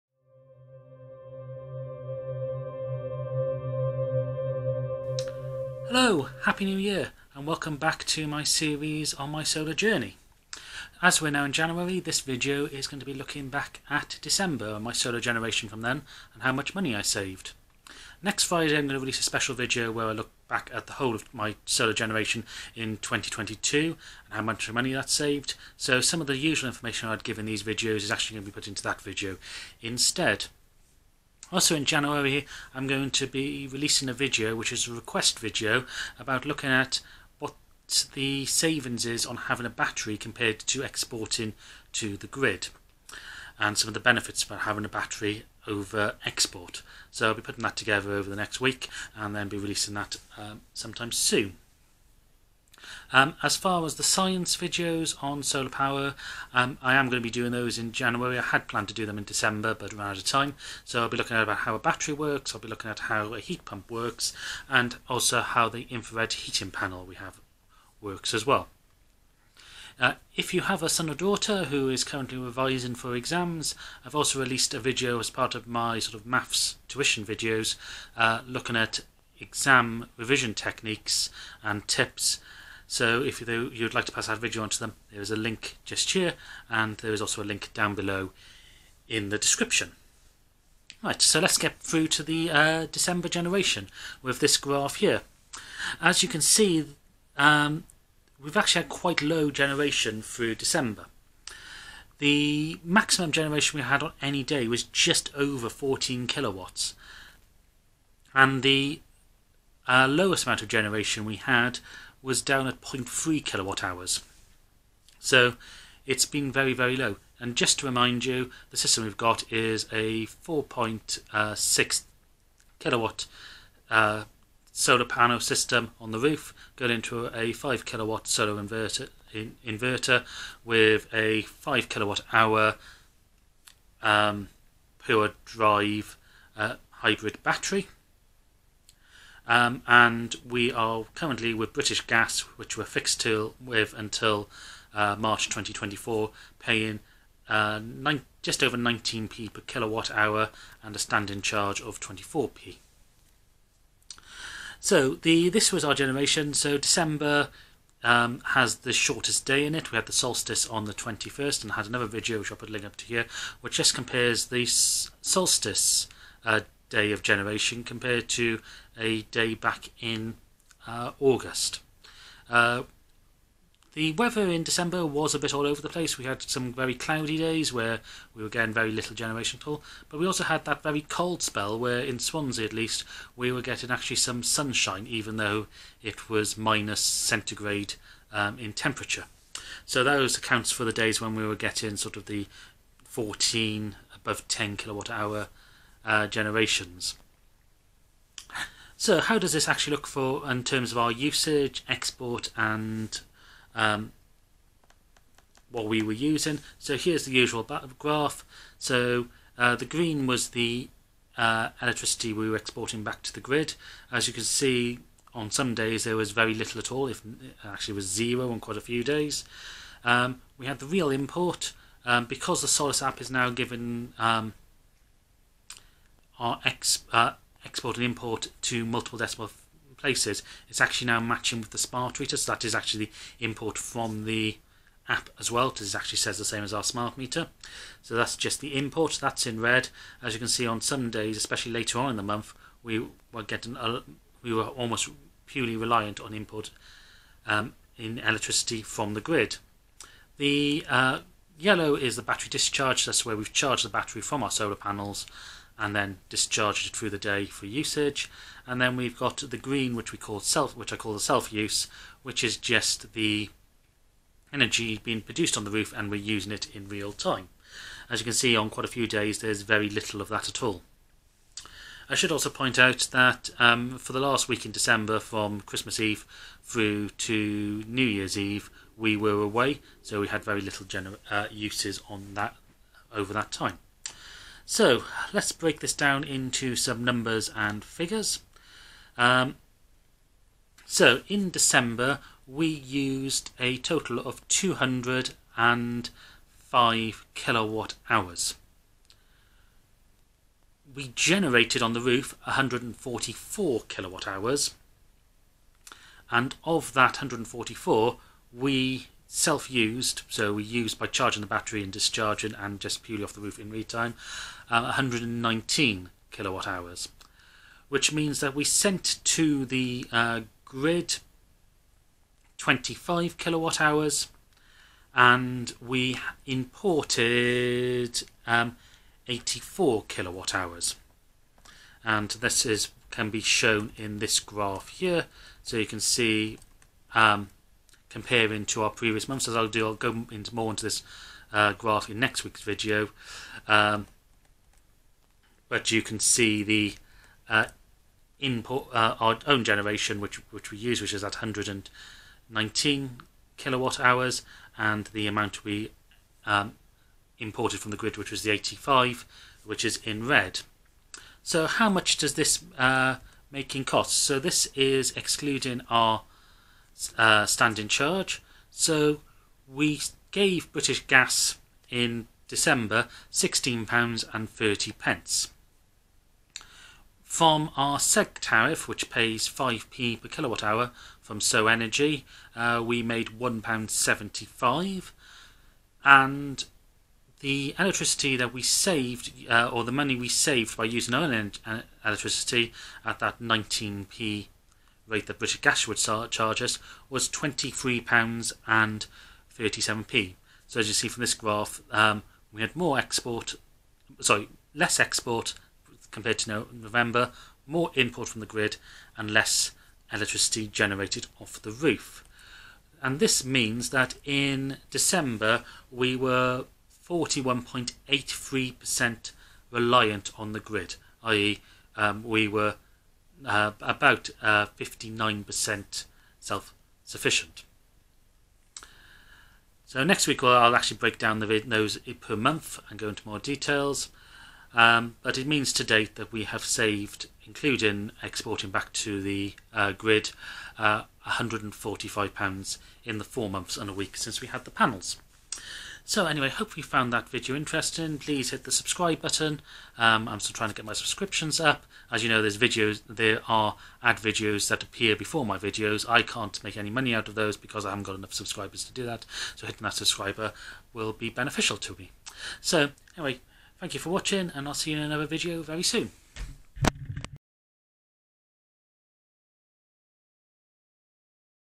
Is December the Worst Month For Solar Panels? My Solar Generation and Savings in December Review
Here is my review of my solar generation and money and energy savings from December.